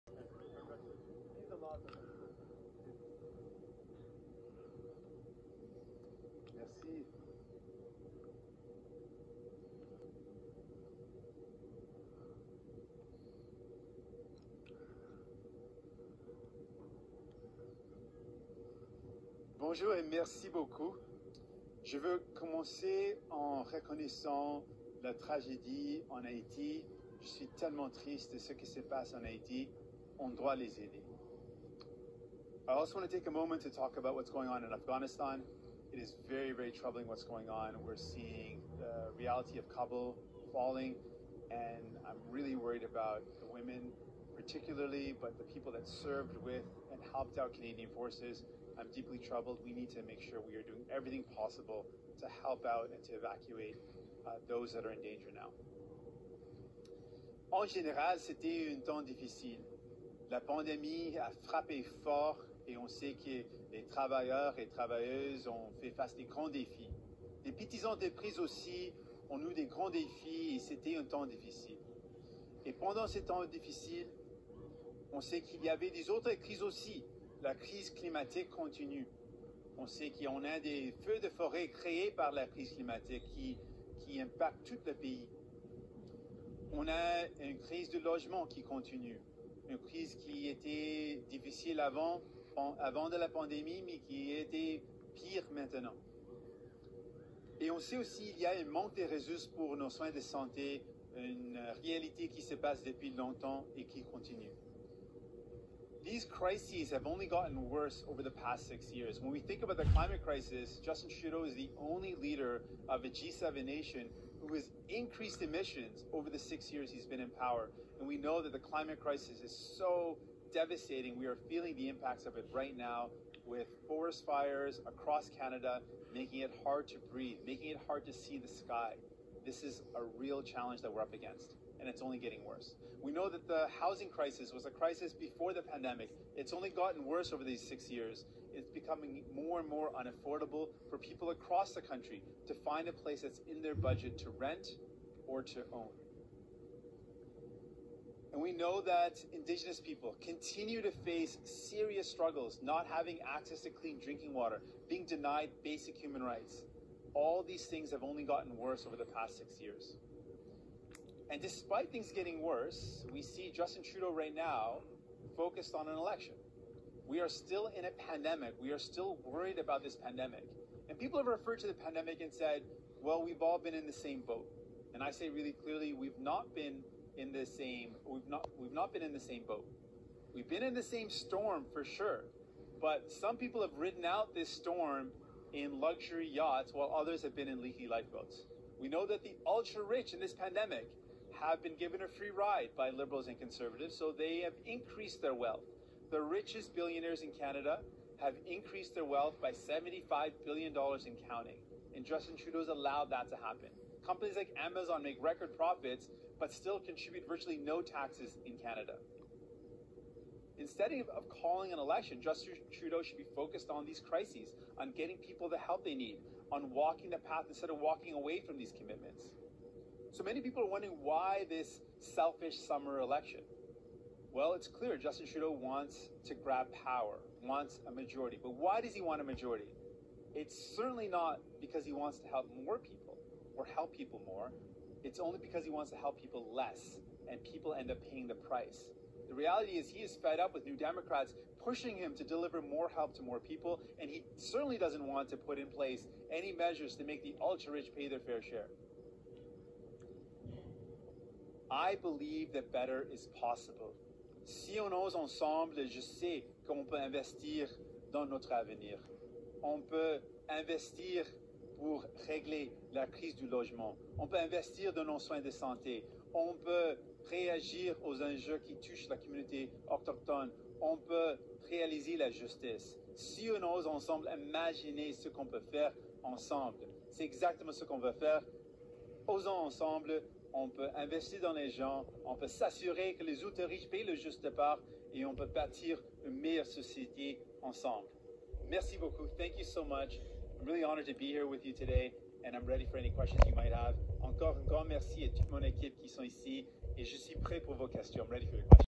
NDP Leader Jagmeet Singh launches election campaign in Montreal, Quebec
In Quebec, at an Montreal park, NDP Leader Jagmeet Singh launches the NDP election campaign for the 2021 Canadian Election, and responds to Liberal Leader Justin Trudeau’s election call, calling the election a “selfish summertime election”